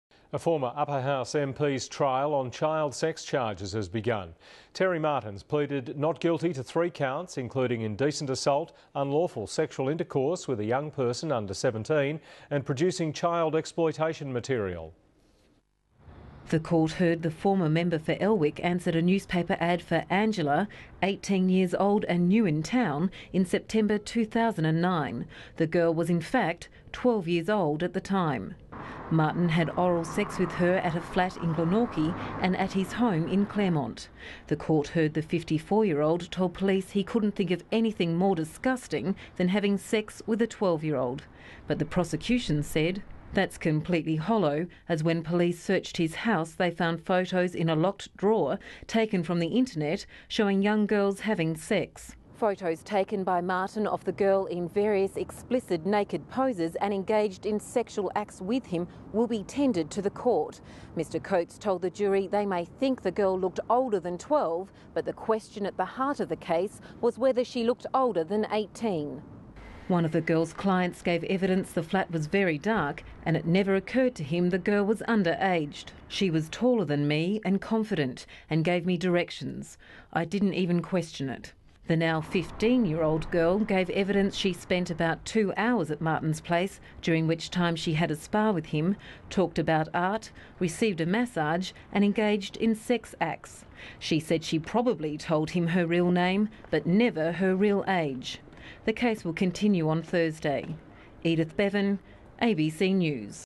Former MP stands trial on child sex charges
A Hobart court has heard former MP Terry Martin believed a girl he engaged for sex through a newspaper advertisement was 18 not 12.